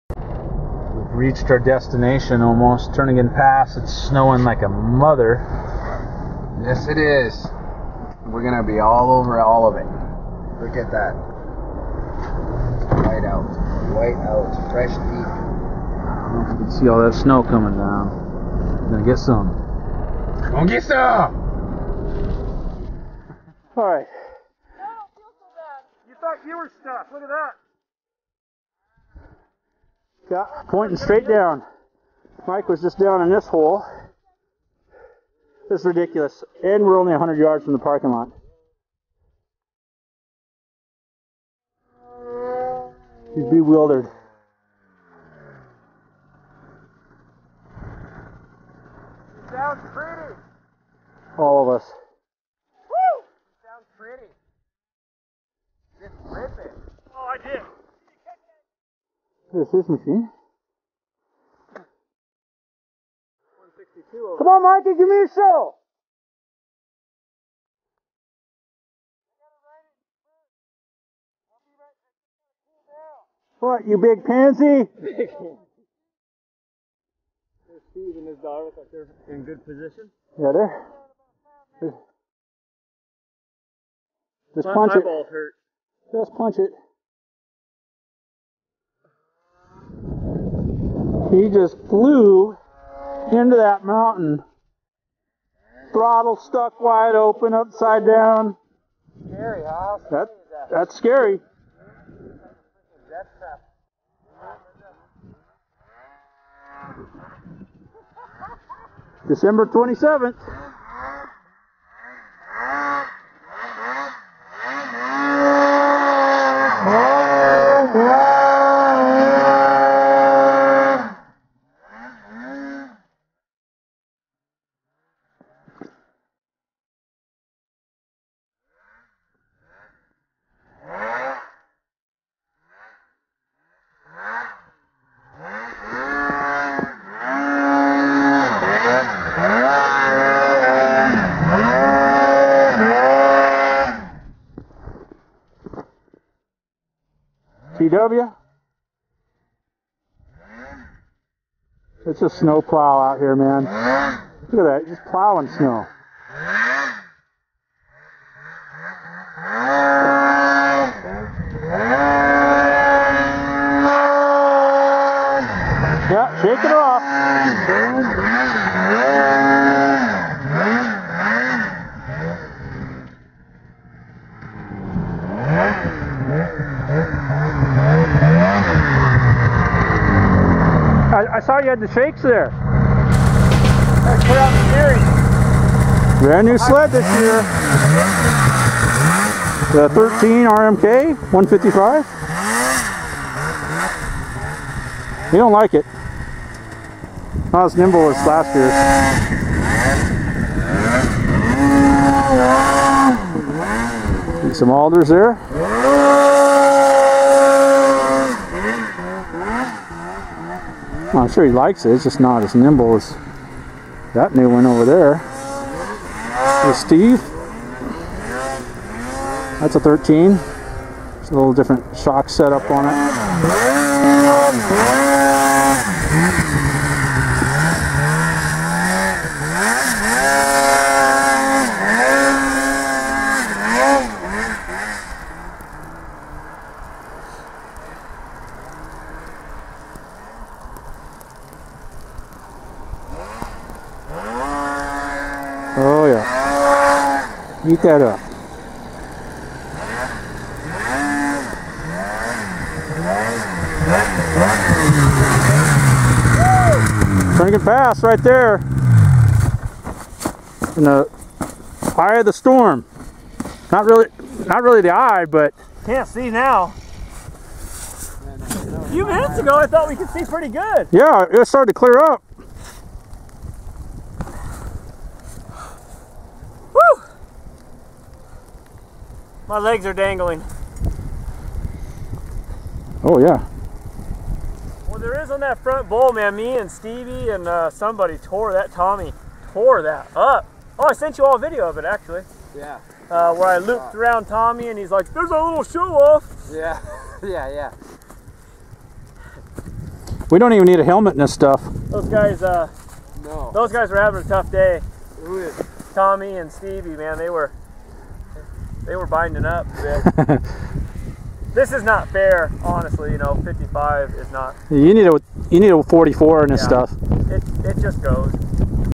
Turnagain Pass Stuck as Hell! Part 1, 2012
Turnagain Pass Alaska, begining of the ride, laying down track and getting stuck as hell!